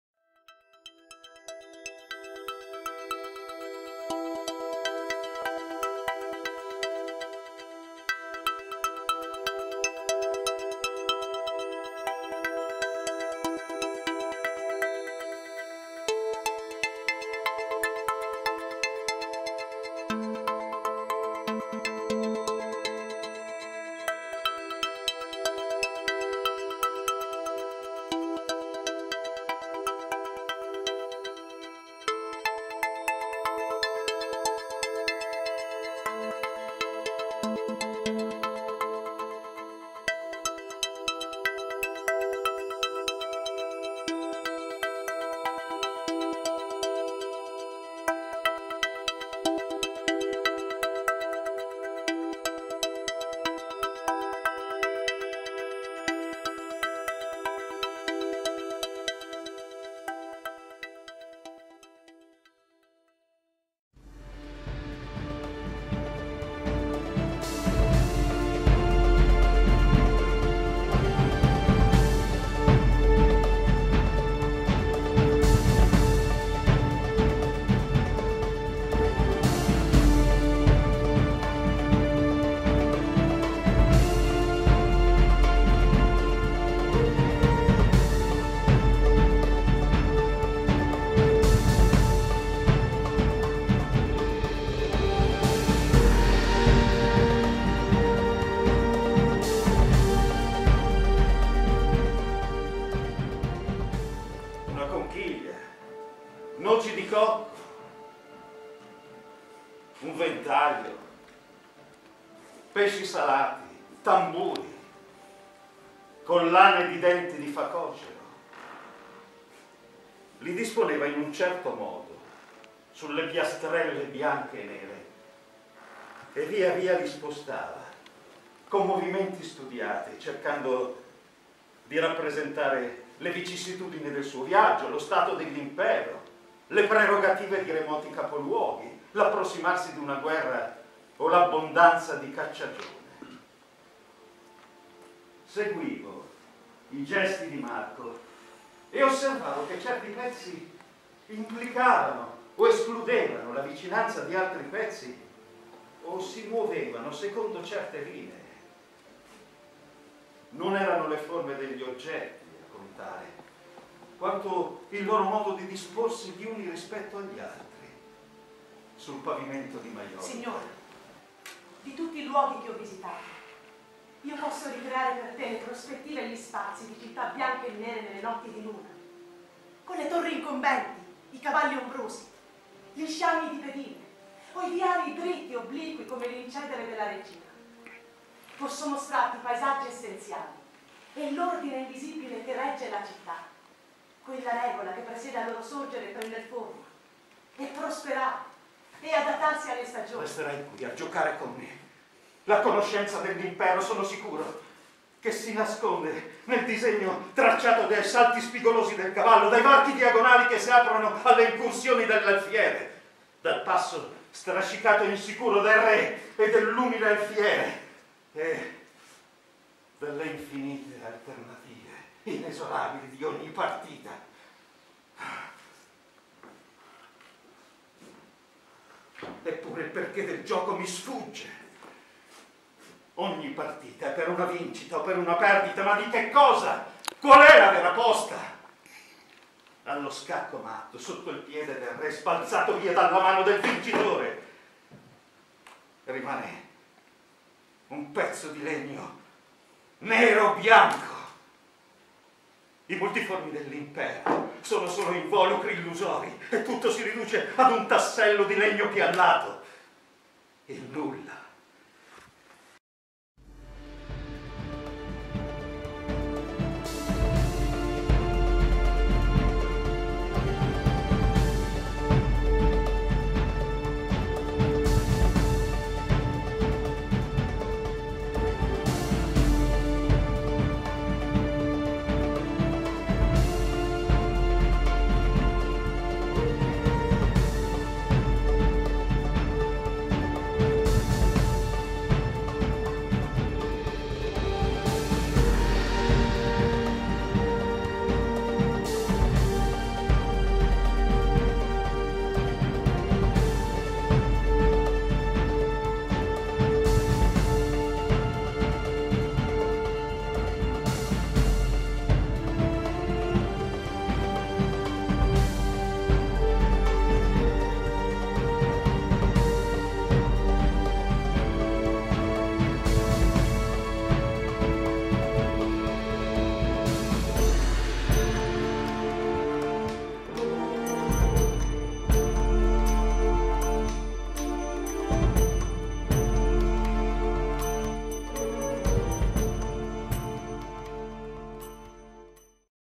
Argilla Teatri ." Le citta' invisibili" di Italo Calvino .Teatro del Quarticciolo .Settembre 2017
"Le citta' Invisibili" di Italo Calvino ,Testi selezionati da Isabella Moroni . In scena al Teatro del Quarticciolo .Roma Settembre 2017